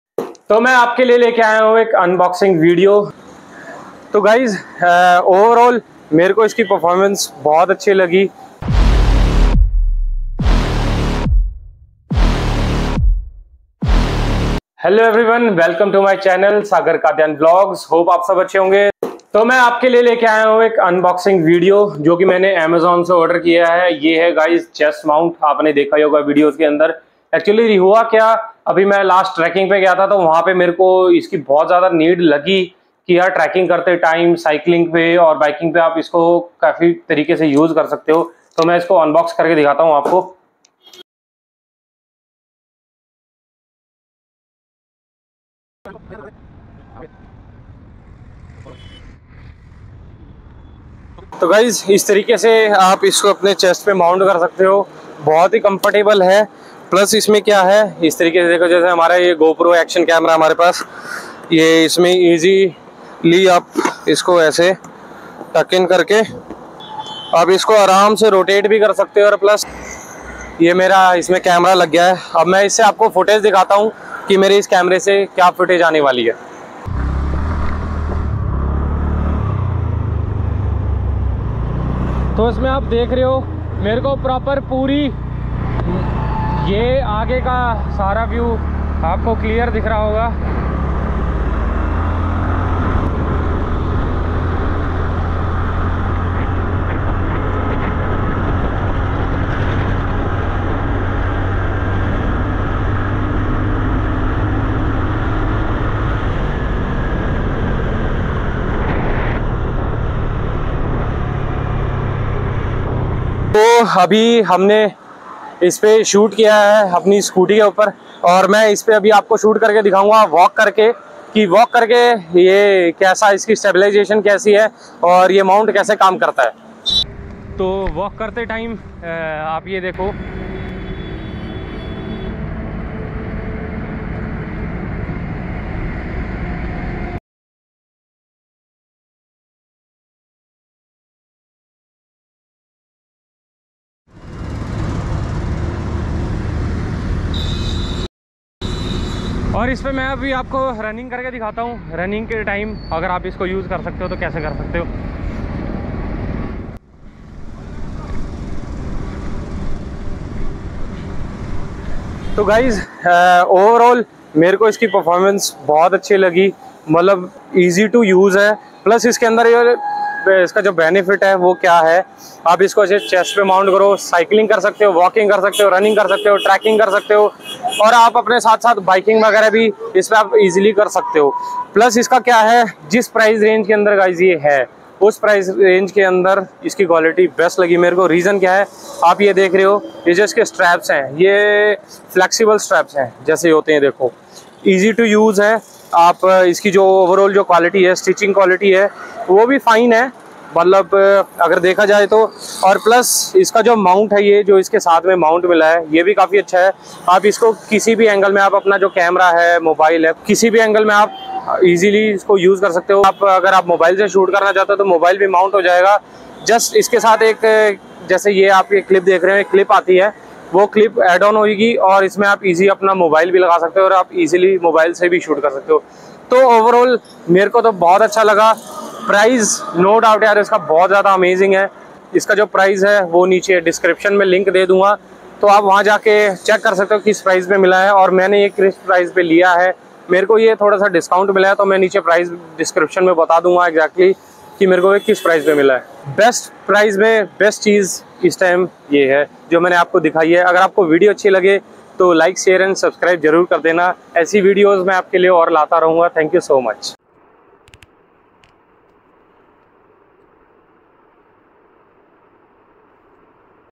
Cheap and Best Vlogging setup | Chest Mount for Mobile and GoPro 🔥 | Most Affordable 😯
Chest Mount:- just ₹259/-

Quikprof Handlebar Plastic GoPro Adjustable Chest Strap Mount Body Belt Harness for Gopro Hero, SJCAM, Yi & Other Action Cameras - Black

Gears that I use :
GoPro Hero 11 waterproof Action camera :